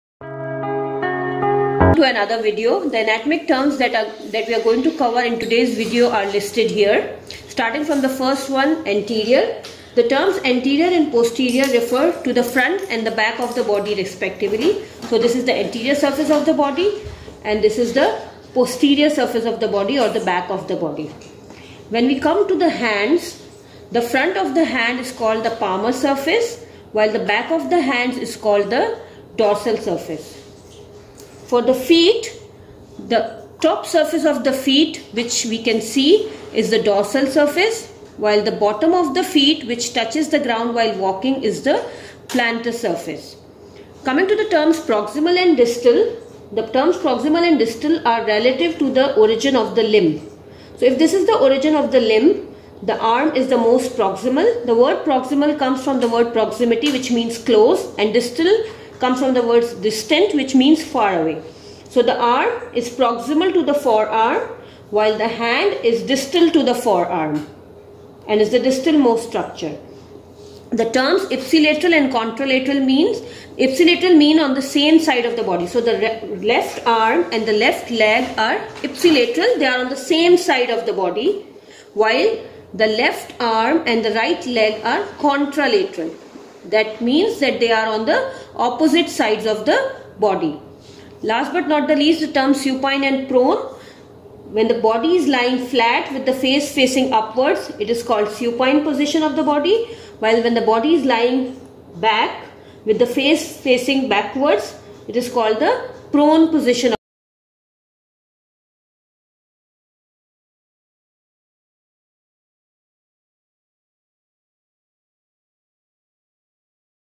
Anatomic terms related to body surfaces explained in 2 minutes
A clear understanding of the basic anatomic terms is a vital and basic step in the journey towards learning medicine. This video of Medicine Made Easy aims to simplify the concept and make it understandable for medical students.
Terms covered in this video:
Anterior Surface
Posterior Surface
Palmar Surface
Plantar surface
Dorsal surface
Proximal /Distal
Ipsilateral/Contralateral
Supine/Prone

Reference Book: Richard S. Snell. Clinical Anatomy by Regions. 9th Edition.